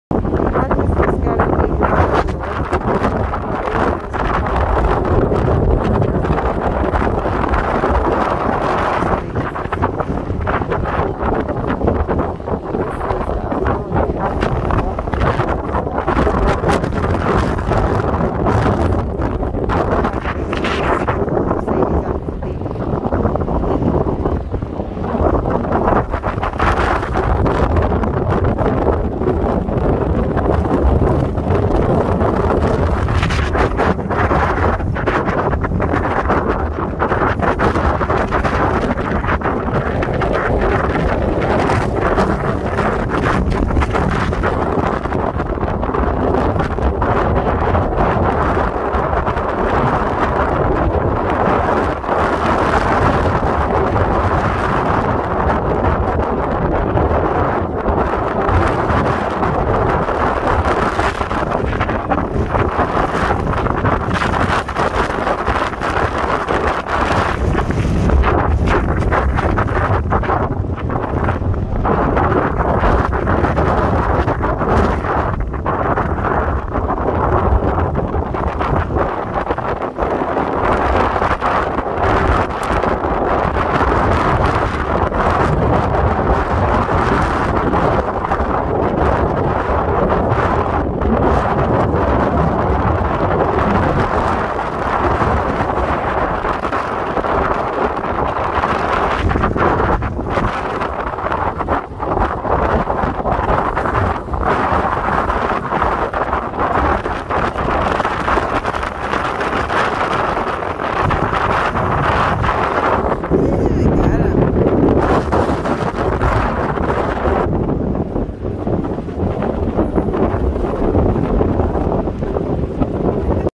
2022 RAV4 Hybrid Recovers Stuck Mercedes From The Beach - Pismo Beach Oceano Dunes
First recovery ever in the new rav. just got the hitch receiver installed.